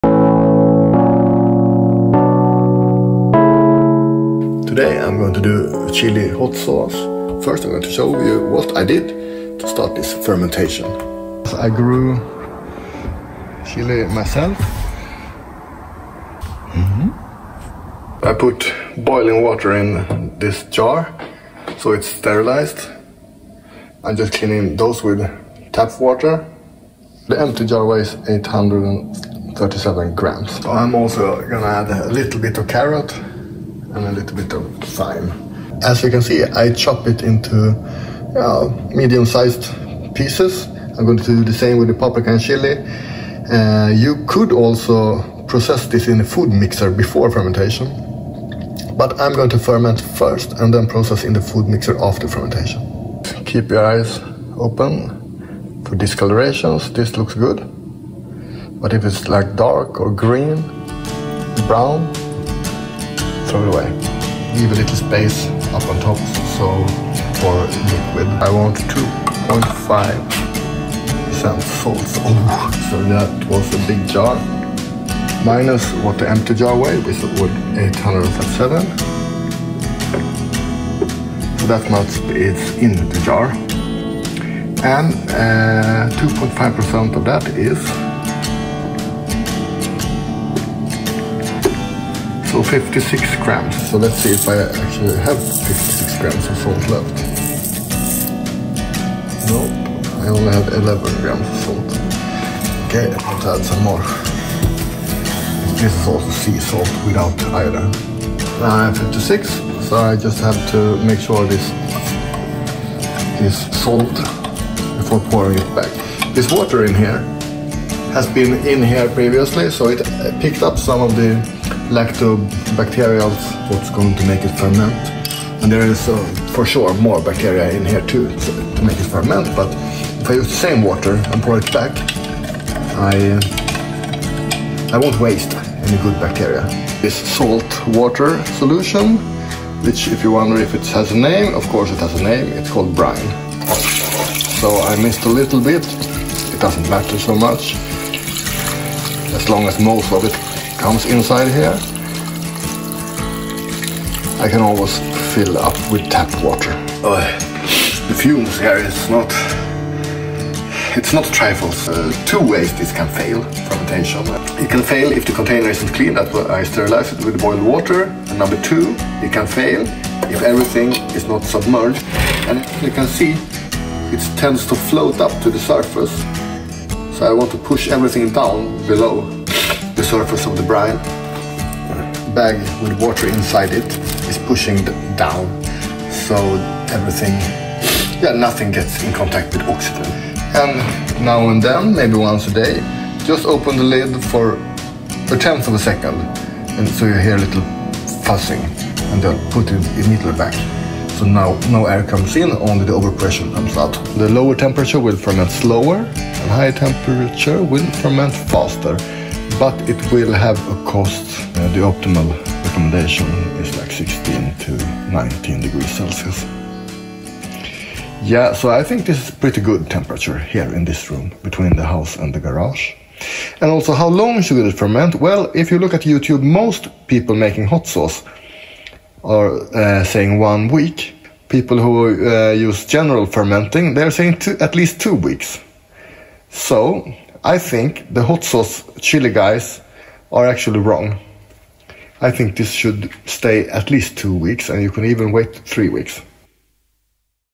Hot sauce part 1 - fermenting
In this video I start the fermentation process. It will stay fermenting for 2 weeks and if everything turns out good I will make the hot sauce 2 weeks from now and probably another video.